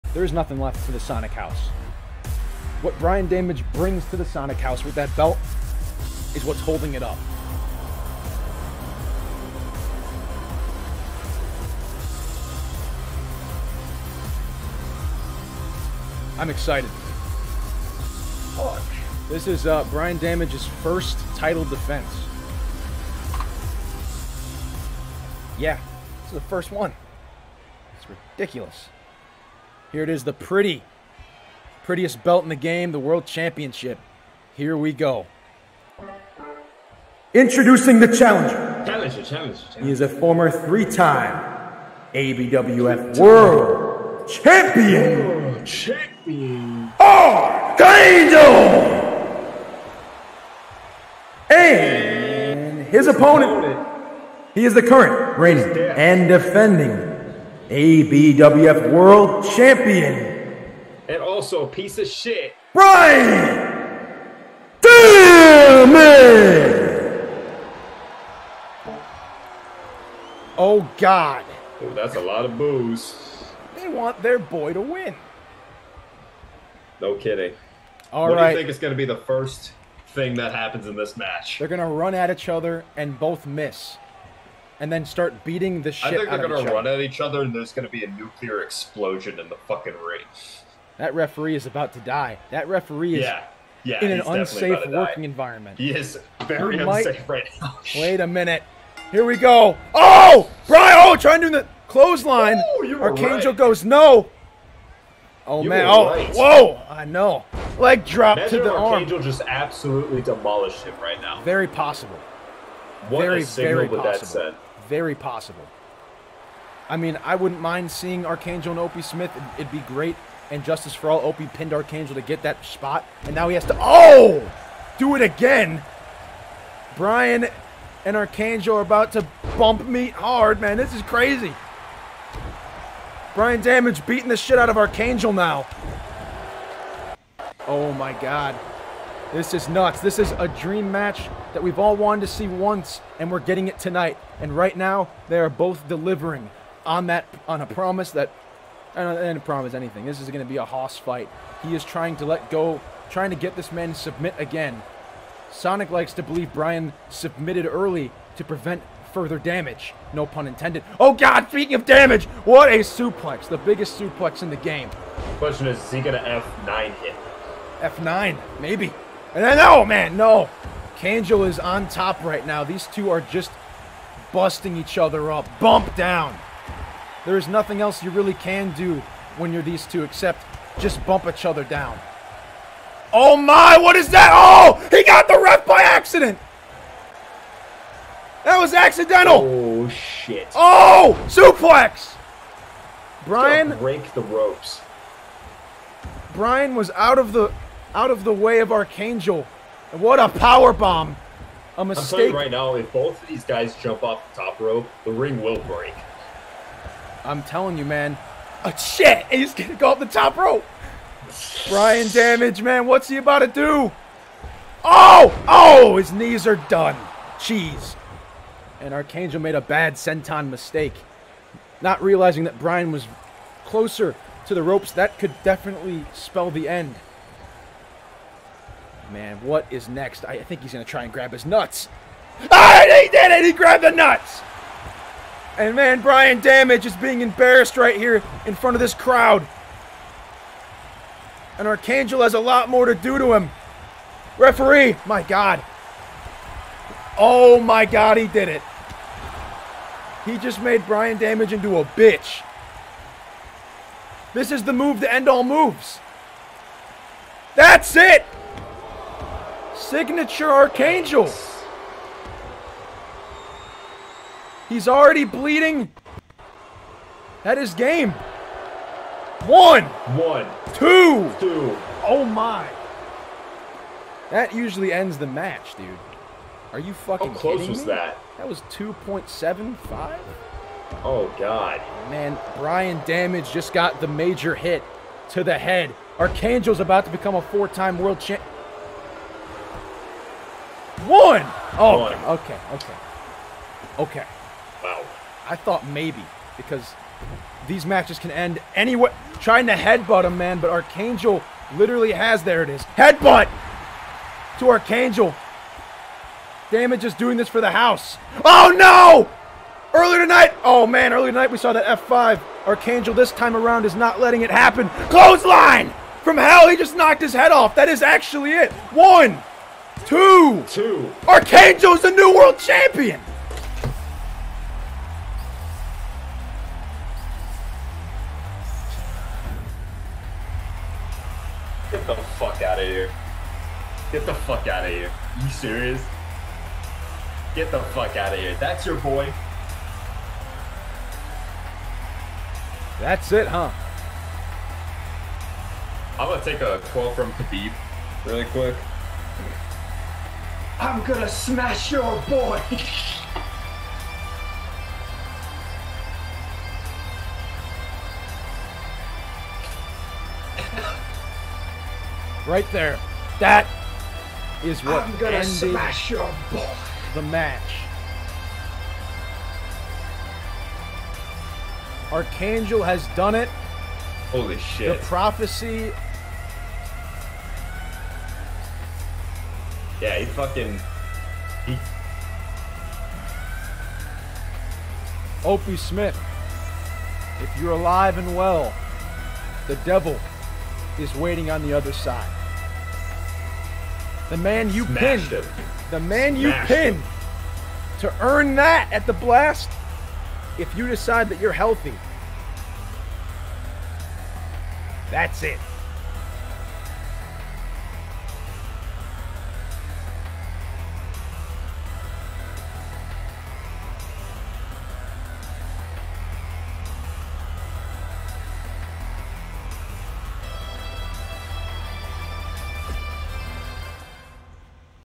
ARCHANGEL V. BRIAN DAMAGE / ABWF WORLD TITLE / NEW 4X CHAMPION CROWNED! - (ABWF Frenzy - 9/4/23)
TONIGHT, WE WITNESS ONE OF THE BIGGEST MAIN EVENTS IN FRENZY HISTORY! ABWF WORLD CHAMPION BRIAN DAMAGE WILL PUT HIS TITLE ON THE LINE AGAINST ARCHANGEL IN THE MAIN EVENT OF A HISTORIC FRENZY AHEAD OF THE BLAST ON PPV! ALSO, MARKO & REBEL KID TEAM UP TO FACE THE EOV, IKEDA GORO DEFENDS HIS NEWLY WON HARDCORE TV TITLE, KATSUMA VS. SANGO, AND MUCH MORE! IT'S TIME FOR A FRENZY!